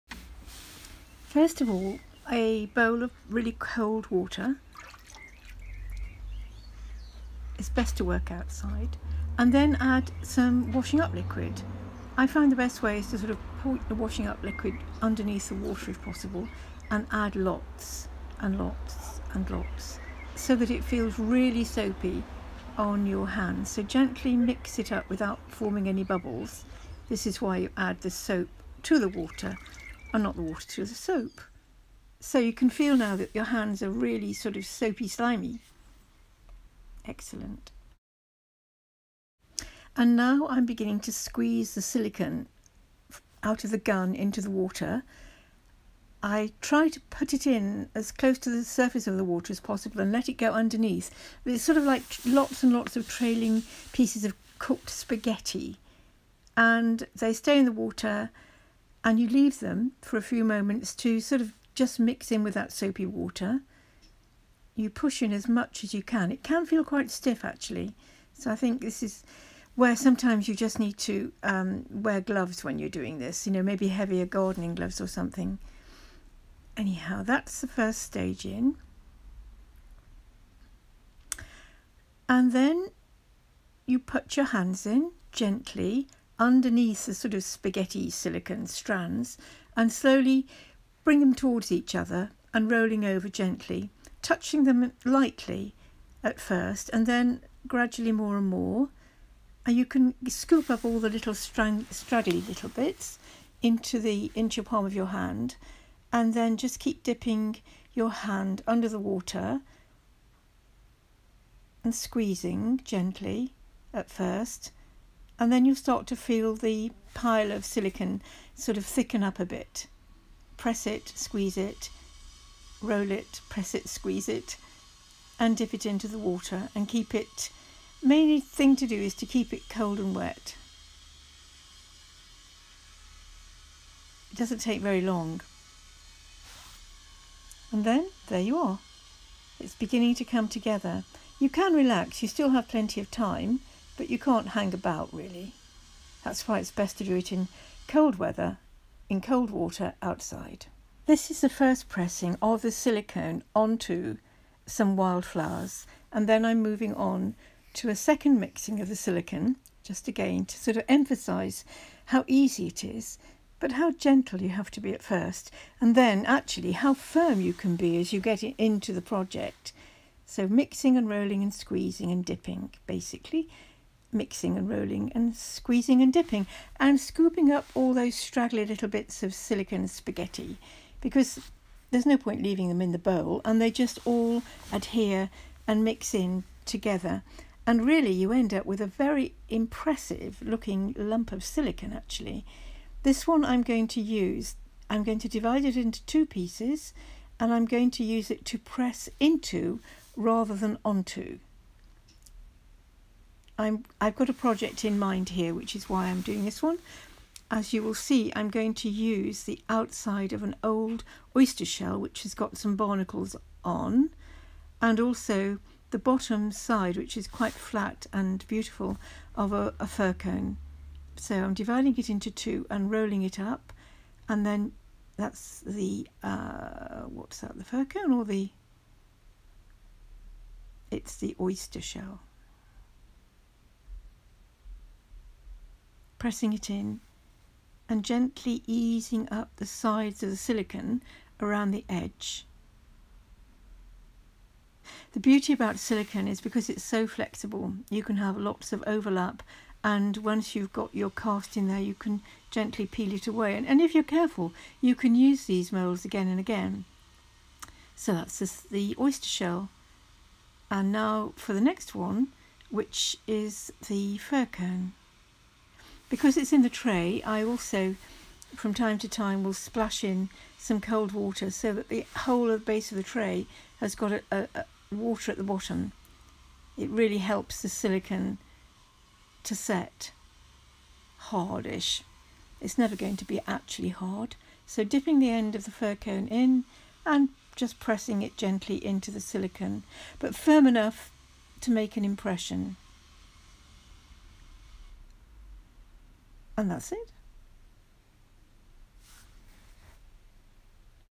The Easiest Way To Make Silicone Molds with Bathroom Caulk.
I prefer to work outside if possible for this method. This is one the easiest way to make silicone moulds with Bathroom Caulk.
If working outside is not an option then this method will make working indoors a whole lot easier and much more pleasant. Keep windows open for some through air flow and a fan on will help.
Silicone Caulk has a vinegary smell - some people don't like it, some don't mind - I don't mind at all. However using a Lemon Scented Washing Up Liquid really helps to lessen and change the smell for the better somewhat - not entirely though.
I’m using 100% Transparent Bathroom Caulk and a Lemon Scented Washing Up Liquid and Cold Water.
I also use a shallow tray to contain the moulds whilst they are curing that I frequently keep splashed with some of the cold soapy water.
As a result the Silicone Caulk does not stick to my hands or to the tray.
If you run out of Silicone Caulk or need to stop for any reason you can get some more - it is inexpensive - and carry on with the project. The new Silicone will stick to previous layer. Not always but mostly.

00.00 First Mixing - adding washing up liquid to a bowl of cold water.
01.25 Gently gathering the silicone spaghetti under the water.
02.11 Continue to work it together out of the bowl.
02.33 Second Mixing.
03.41 Dividing the Silicone in to two pieces.
03.52 Pressing in the old Oyster Shell.
04.28 Pressing in the Fir Cone base.

Ideally an outdoor space - if not perhaps by an open window or try to create a
through-draught.  or put on a fan to disperse any fumes.

Fair sized washing up bowl or similar.
Cold water.
Washing up liquid.
A plastic tray.
Clear Silicone Caulk.
Objects of your choice - natural and/or found.
A little time and patience.